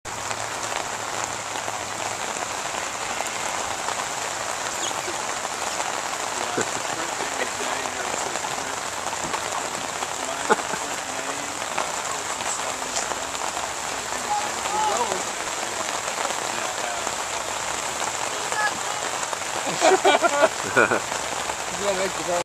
Boy Scouts Canoeing in the Ditch
When it rains it pours and the Boy Scouts were able to take their canoe demonstration out of the canal and into the ditch.